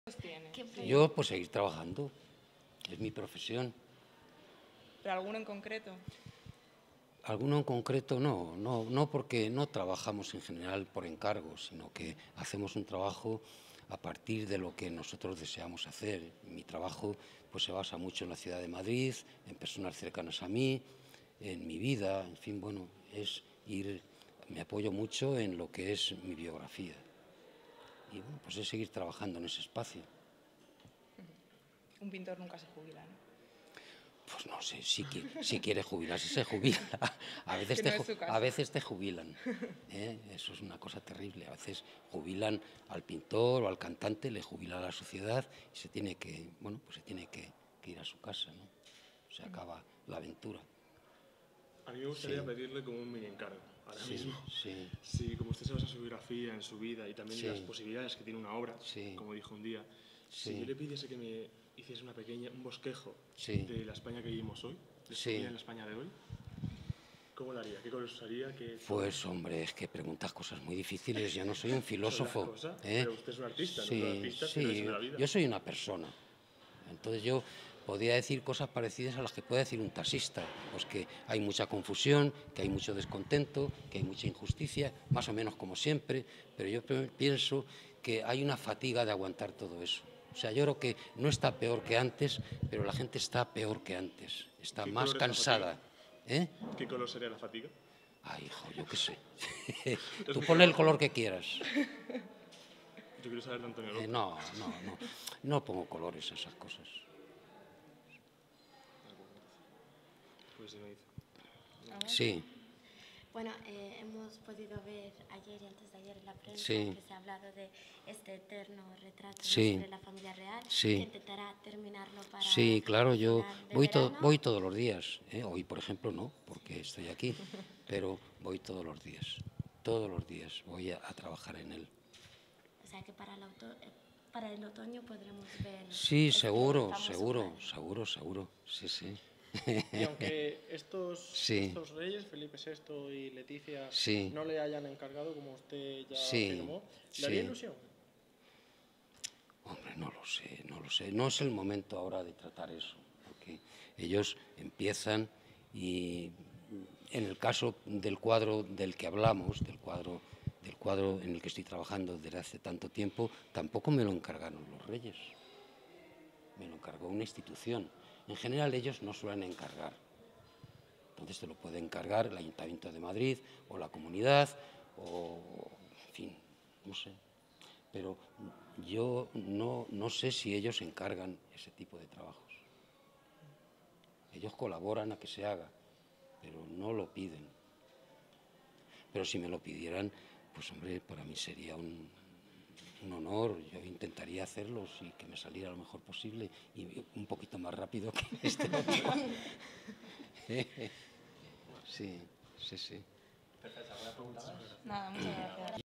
Antonio López en los cursos de verano UCM 2014
Rueda de prensa de ANTONIO LÓPEZ(3 de julio del 2014) en los CURSOS DE VERANO de la UNIVERSIDAD COMPLUTENSE DE MADRID